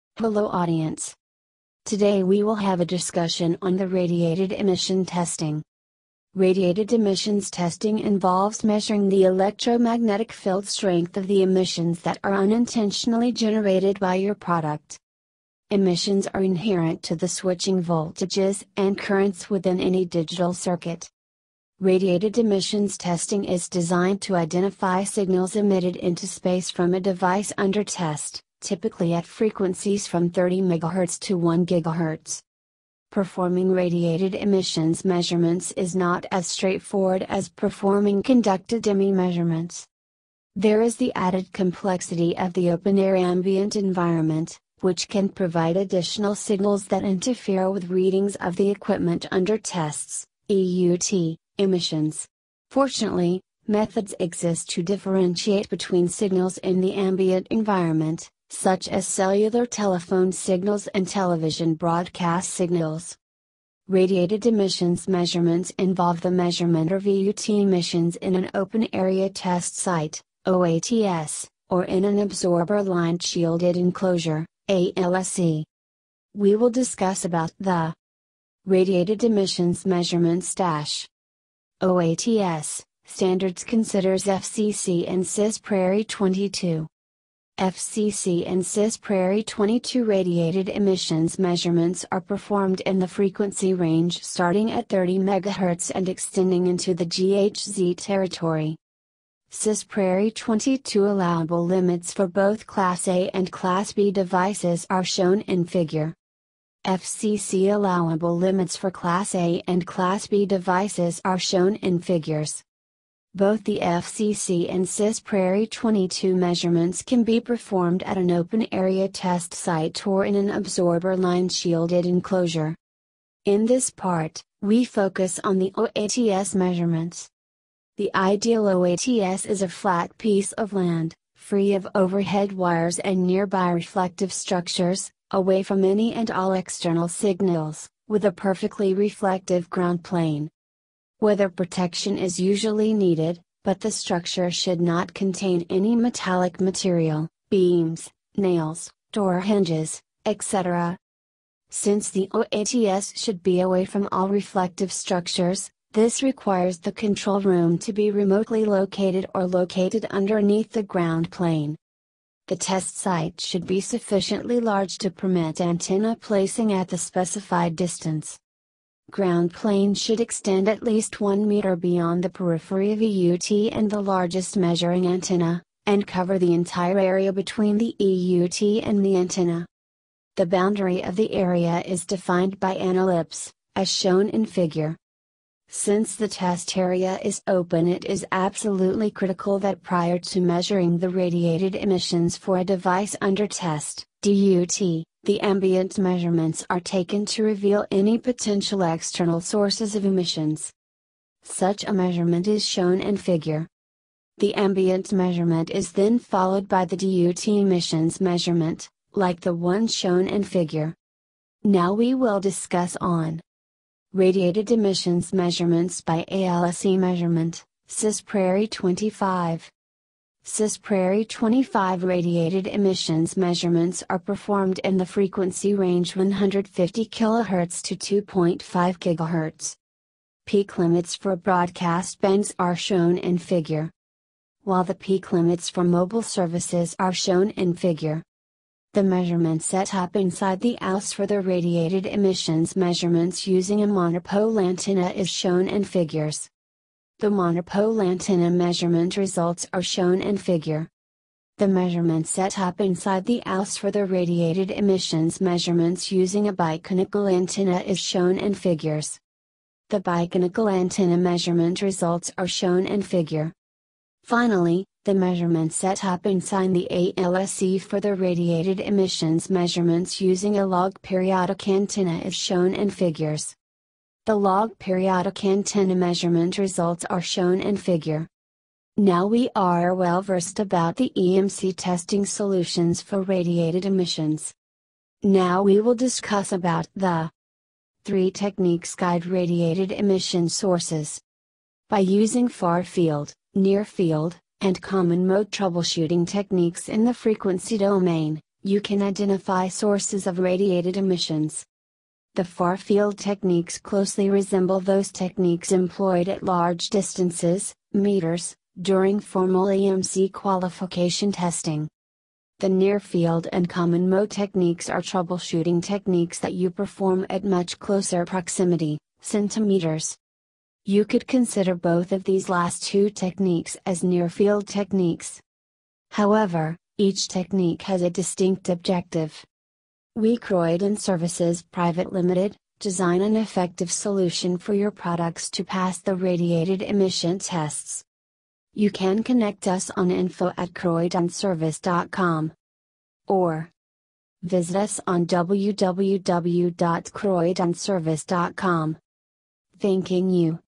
Why do we have multiple ways to measure Radiated electromagnetic Noise?
Electromagnetic noise radiated from electronic devices can be measured and evaluated using a handful of techniques, yet the EMC standards define methods very selectively based on product type, applicable industry and several other significant parameters, this video give a first hand overview of Radiated emission measurement.